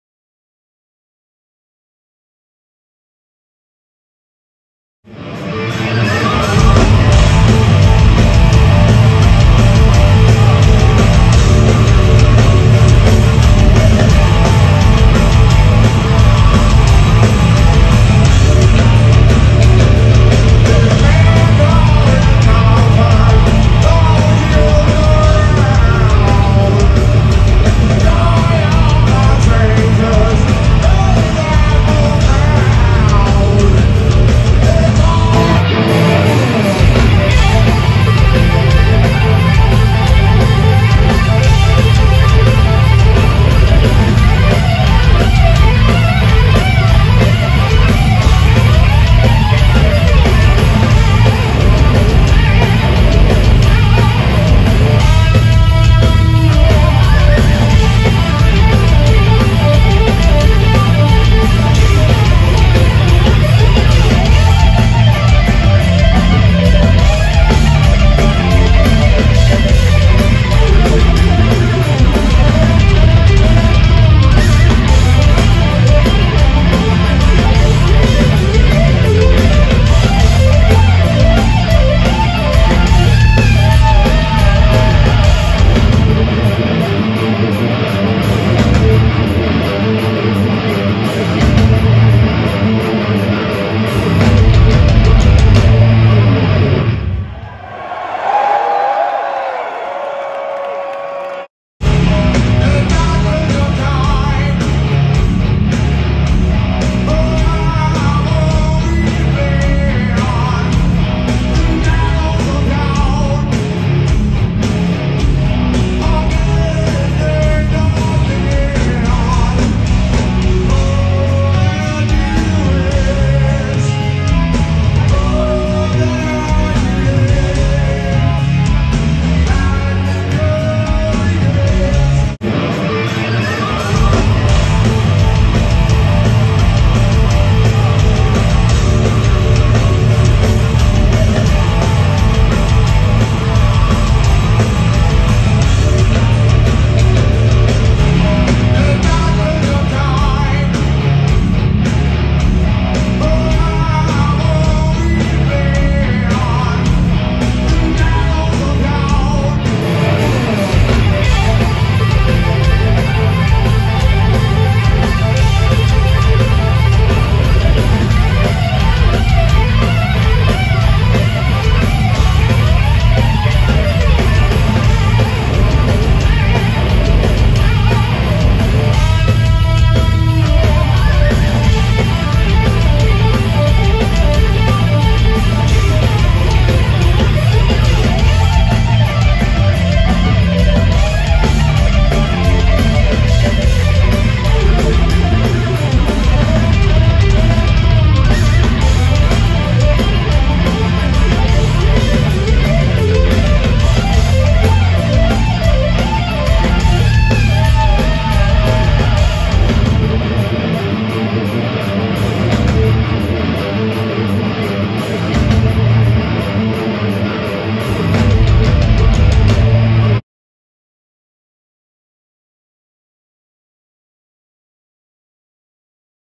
Black Label Society 2/8/23
Photos, Video and Audio from blacklabelsociety in St. Charles, MO 2/8/23 on Tour with anthrax and exodus